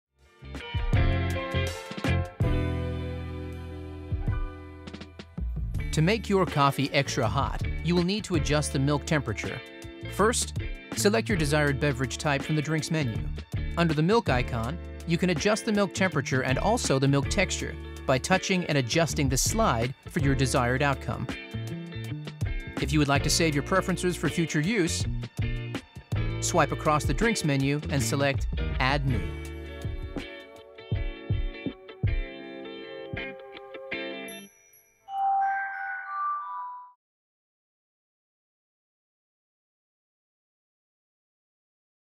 The Barista Touch™ | A guide to adjusting the milk texture of your coffee | Breville USA
Say hello to smooth, velvety milk textures with the Barista Touch™.

Great tasting coffee starts with the right milk texture. Be hands on like a barista and control the temperature and texture of your milk to achieve great tasting coffee with an irresistible body, all thanks to the Barista Touch™.

Directions:
1. Select your desired beverage type from the ‘Drinks’ menu.
2. Under the ‘Milk’ icon, select ‘Temp’ to adjust the milk temperature and ‘Froth’ to adjust the milk texture.
3. Tap and adjust the side for your desired outcome.

To find out how to clean and unblock the steam wand of your espresso machine, watch